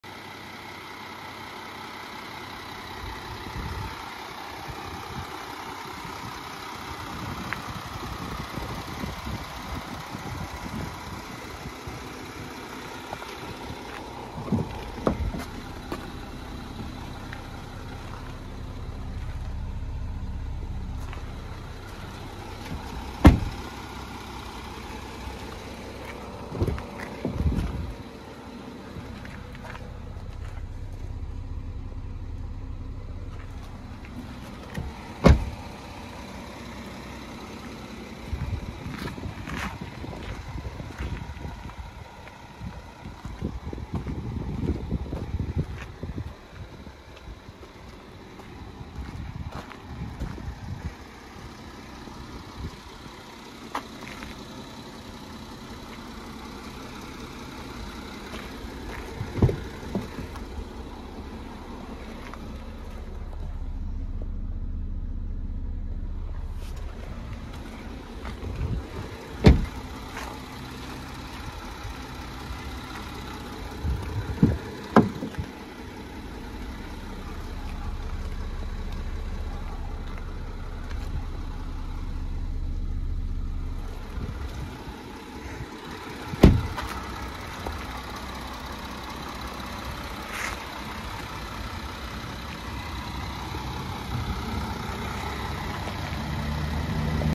Volkswagen Amarok 3.0 TDI V6 Aventura ( AF19 KRO )
£32990 + VAT. NATIONWIDE DELIVERY SERVICE AVAILABLE ** ONE OWNER. FULL VW SERVICE HISTORY. 2 SERVICES. JUST BEEN SERVICED AT 34K IN 04/2023. SAT/NAV. FULL LEATHER INTERIOR, ERGO COMFORT SEATS, FRONT ELECTRIC AND HEATED SEATS. CLIMATE/CRUISE CONTROL. ELECTRIC WINDOWS AND MIRRORS, ELECTRIC HEATED AND FOLDING MIRRORS, PADDLE SHIFT. DIFF LOCK, STOP/START.